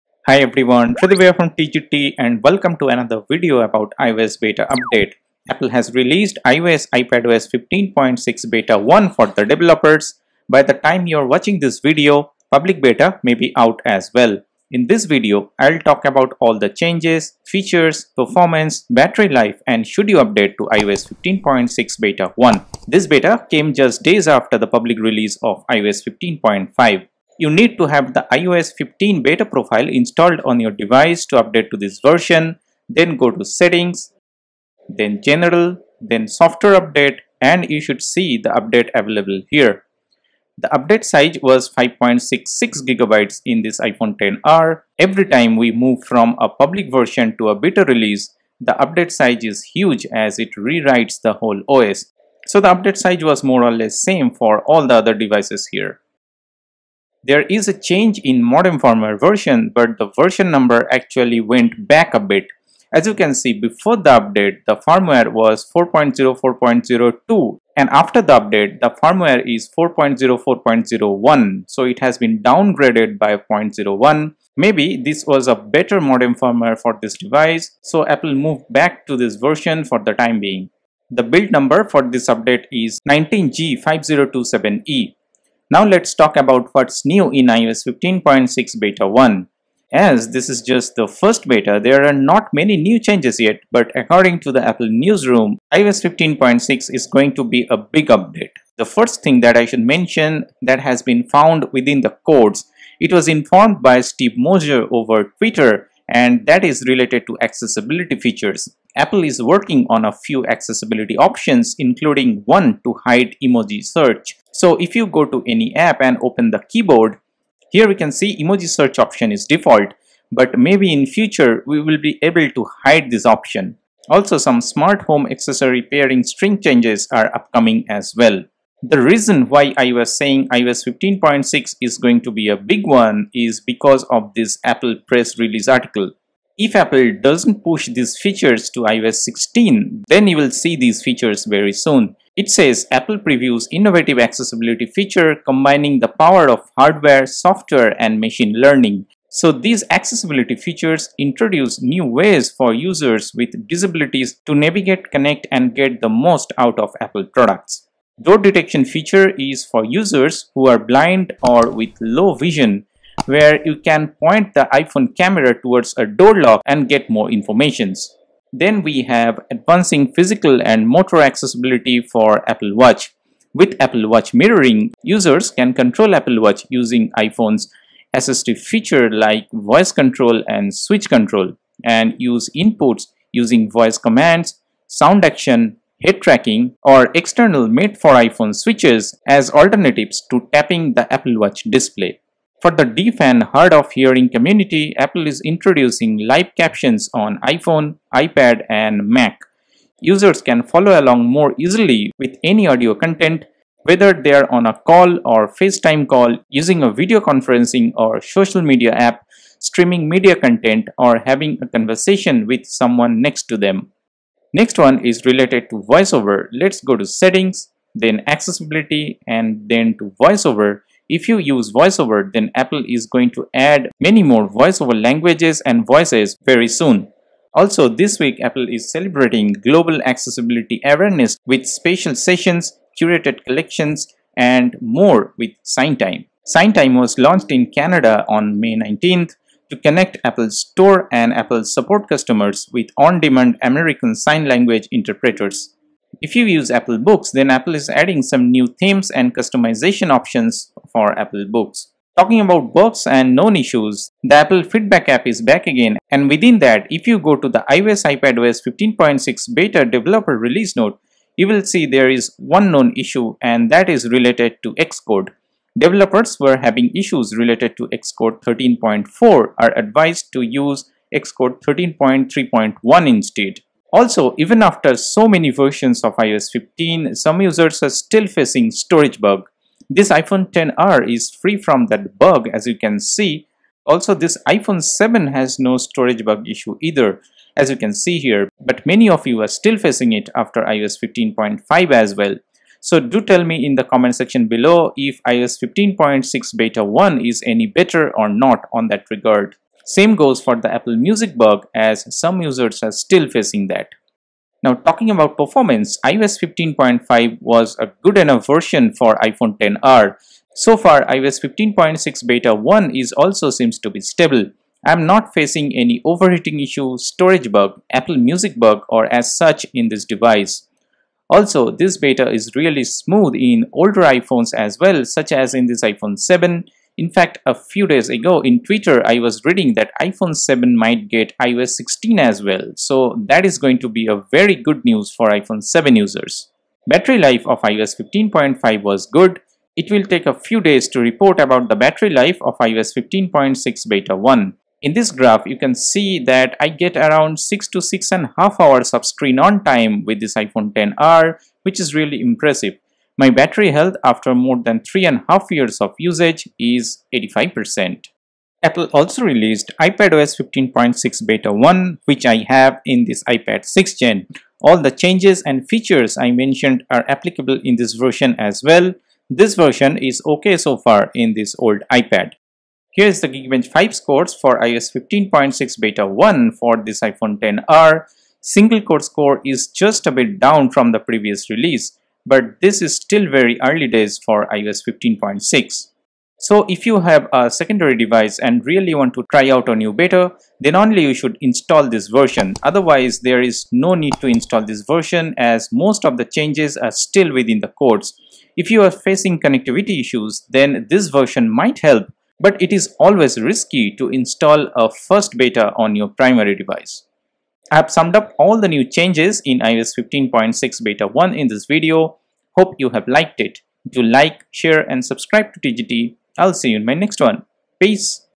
iOS 15.6 Beta 1 Review | Performance, Battery life, Should You Update | TGT
Apple has released iOS 15.6 Beta 1 for developers and beta testers around the world. iOS 15.6 hints at some new upcoming features such as, changes within code, hide emoji search, new accessibility features, changes in apple books and more. In this video I am talking about all the features, changes, bug fixes, performance & battery life of iOS 15.6 Beta 1 using an iPhone XR, iPhone 7, iPhone 6s and iPad 6th Gen.
Along with that apple also released. iPadOS 15.6 Beta 1, macOS Monterey 12.5 Beta 1, watchOS 8.7 Beta 1, tvOS 15.6 Beta 1 and HomePod OS 15.6 beta 1.
-Chapters-
0:00 - Introduction
0:05 - iOS 15.6 Beta 1 is here
0:27 - How to update
0:40 - Update size
0:56 - Modem firmware & Connectivity
1:21 - Build Number
1:25 - Updates, Changes & New Features
4:12 - Bugs & Known issues
4:34 - Storage Bug & Music Bug
5:04 - Performance
5:37 - Battery Life
5:58 - iPadOS 15.6 beta 1
6:12 - Benchmark scores
6:26 - Should you update
6:47 - Share your thoughts